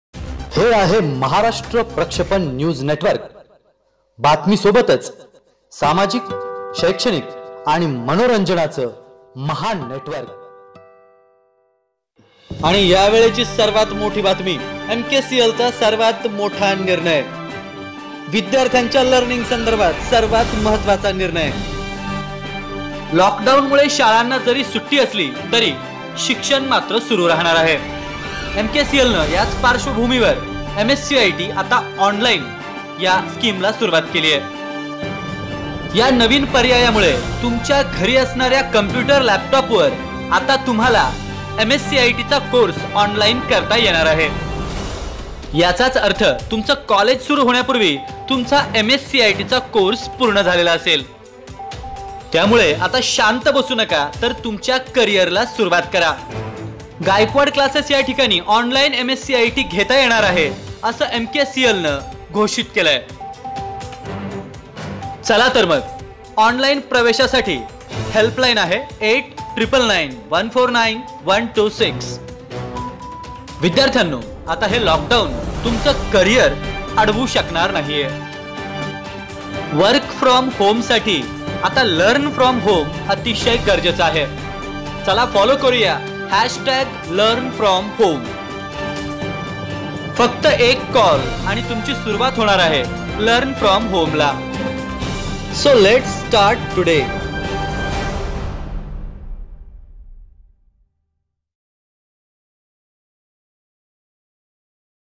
ऑनलाईन लर्निंग ला झाली सुरुवात : तुम्ही केली का नोंदणी | महाराष्ट्र प्रक्षेपण
विद्यार्थ्यांसाठी महत्त्वाची बातमी !
लॉकडाउन च्या परिस्थिती मध्ये वर्क फ्रॉम होम साठी लर्न फ्रॉम होम ला सुरुवात करा.